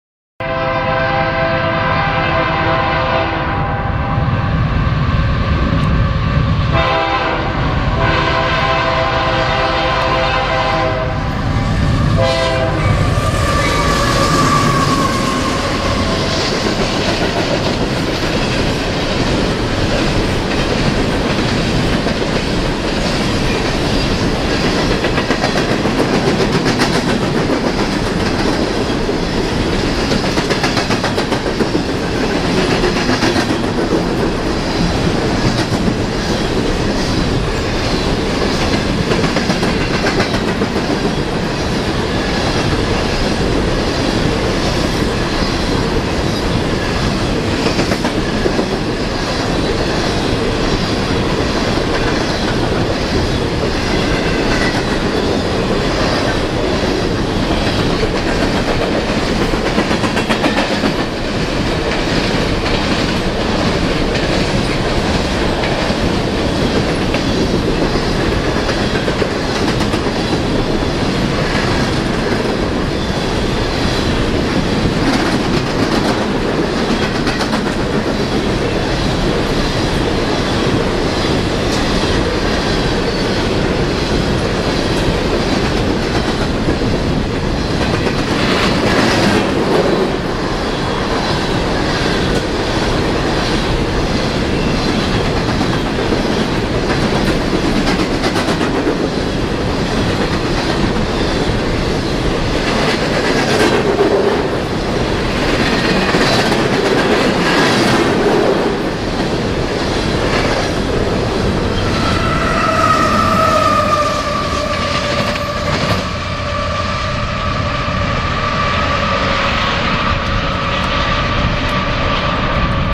More coal on the k line at old monroe nov19, 2010
northbound coal emptey at 7:01pm heading for elsberry 14.8 miles north.